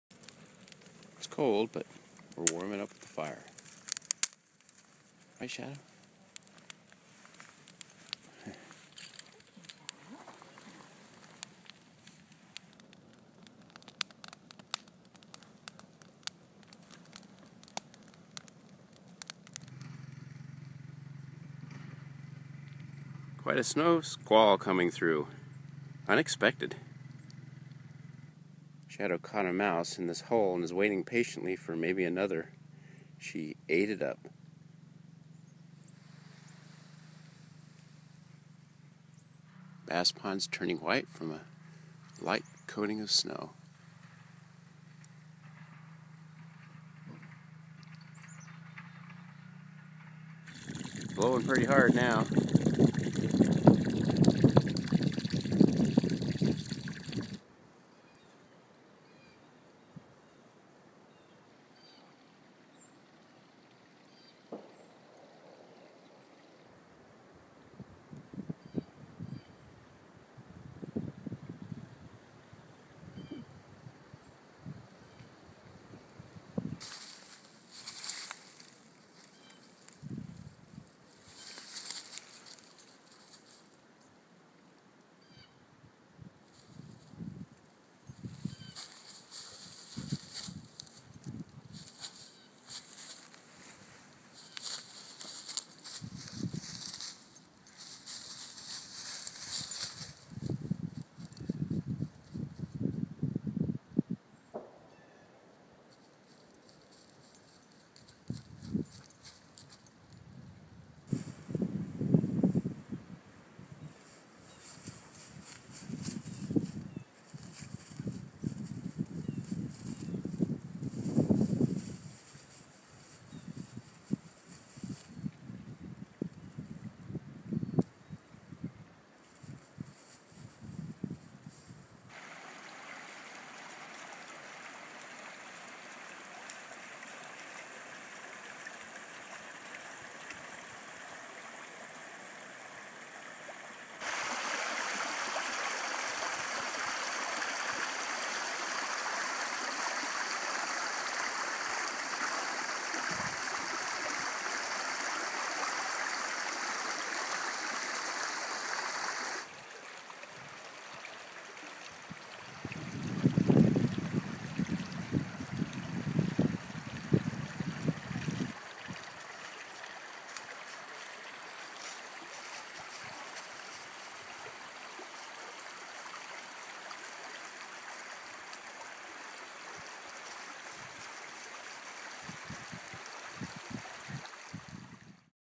Shadow's Weekend Getaway - The best time of year for our Norwegian Elkhound
Video clips that feature our pet dog, Shadow, a 5-year old Norwegian Elkhound hunting and exploring the meadows, streams and ponds during a cold weekend in early spring at our Hidden Hollows property in West Virginia.